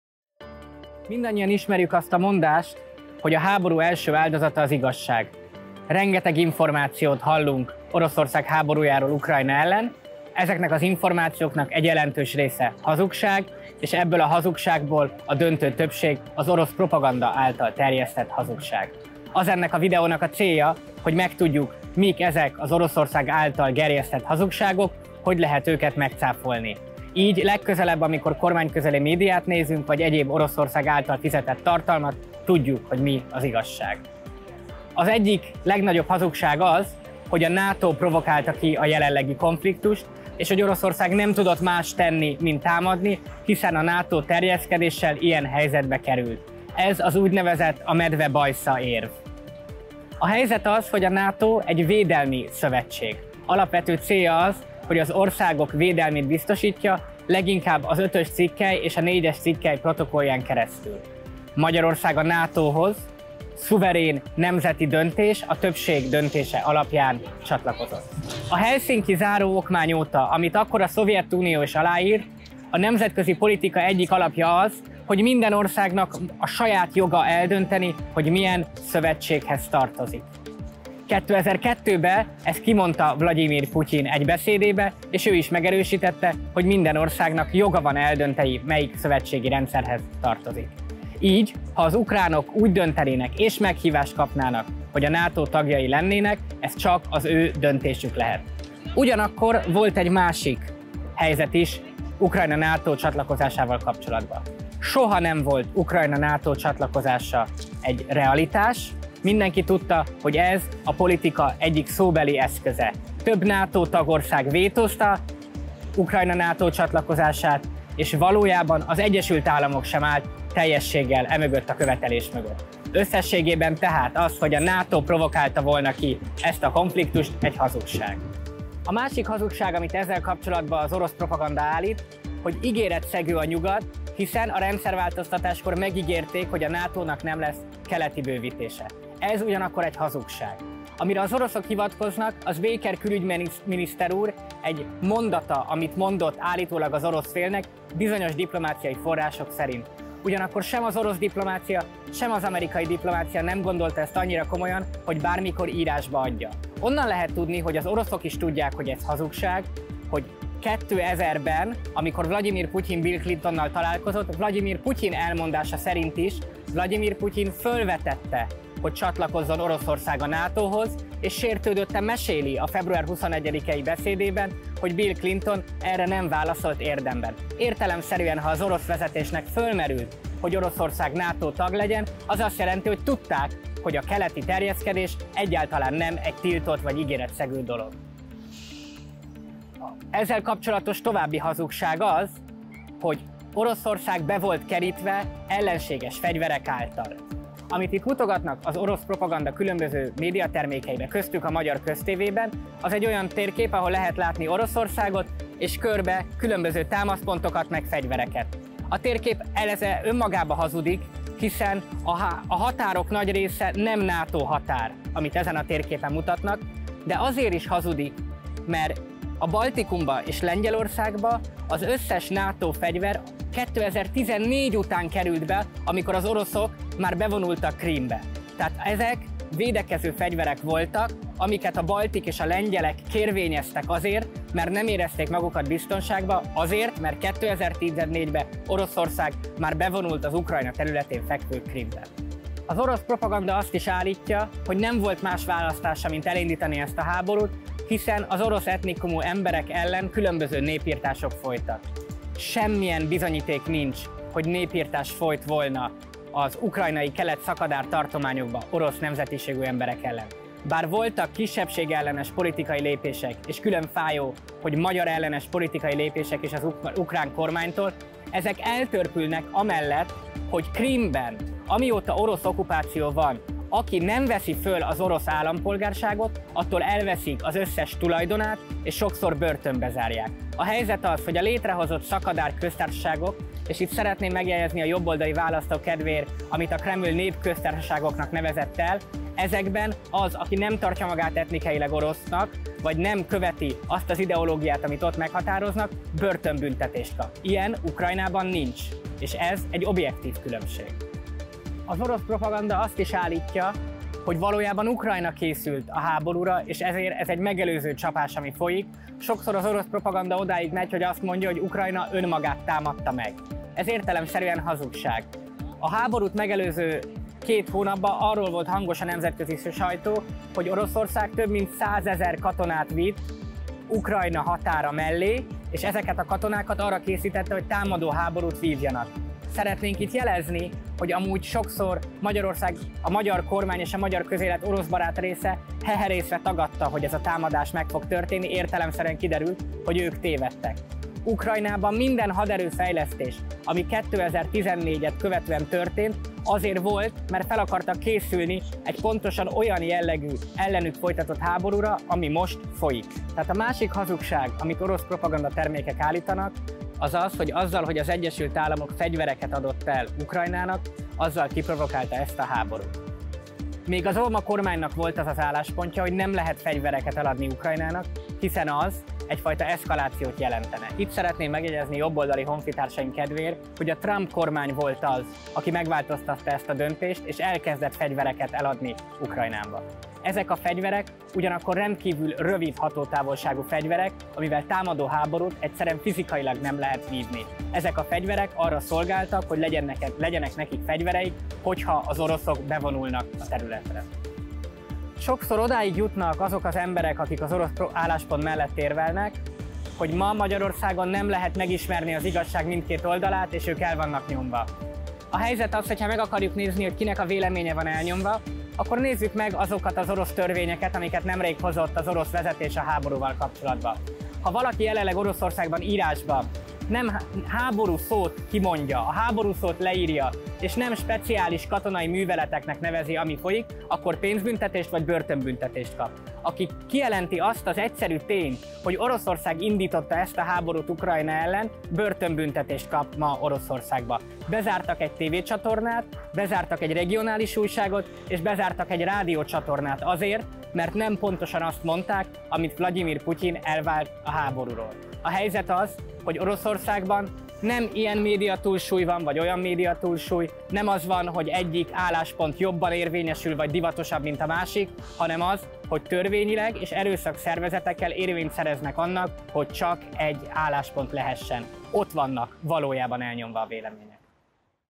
Mi igaz az orosz propagandából és mi nem | Ungár Péter válaszol
Oroszország Ukrajna ellen indított háborújáról rengeteg információt hallunk, aminek jelentős része hazugság. Ezeknek a hazugságoknak döntő része pedig az orosz propagandát szolgálja.
Ungár Péter a fő hazugságokat leplezi le ebben a videójában.